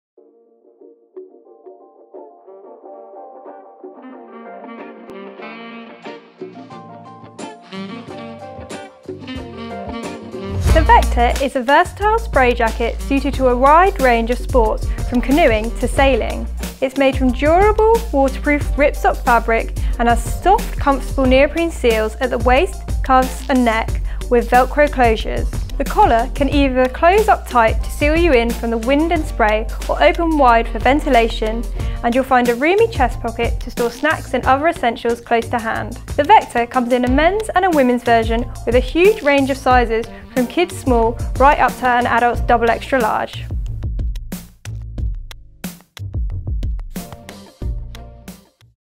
Vector jacket from Palm Equipment
The Vector is a super simple jacket to protect you from wind and spray. Canoeing, kayaking, sailing, or SUP,  it's a great jacket for a range of watersports.